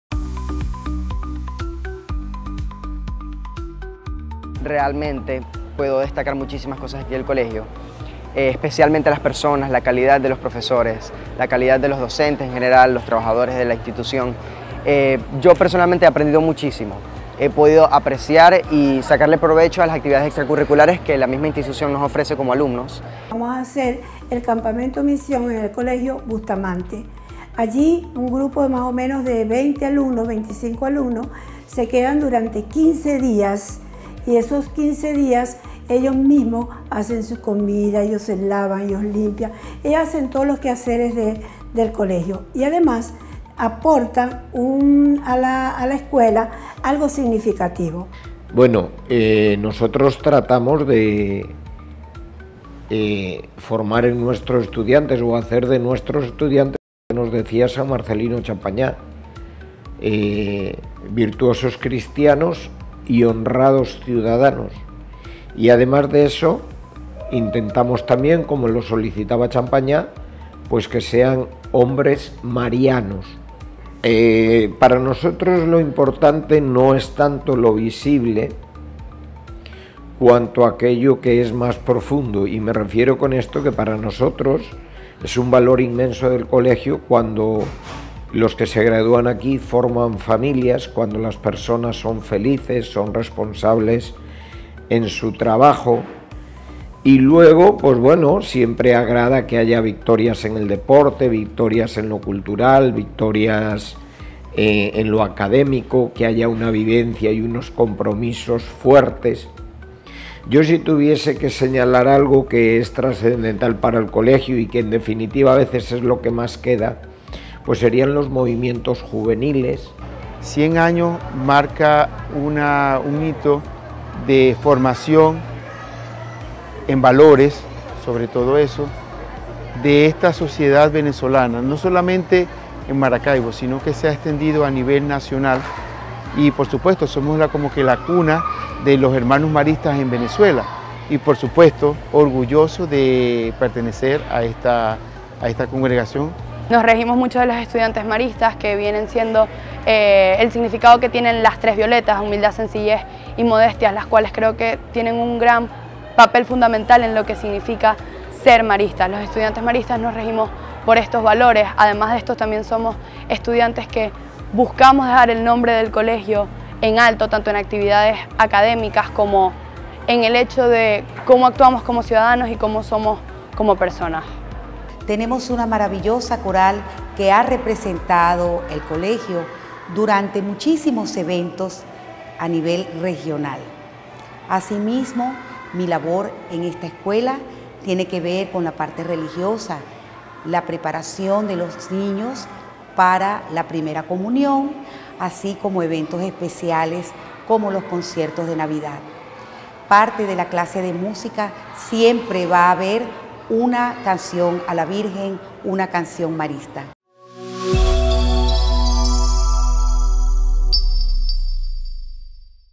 Colegio Nuestra Señora de Chiquinquirá - Maracaibo, Venezuela
El 5 de octubre de 1925 inició clases el Colegio Nuestra Señora de Chiquinquirá en Maracaibo, Venezuela, siendo su primer director el Hno. Ildefonso Gutiérrez.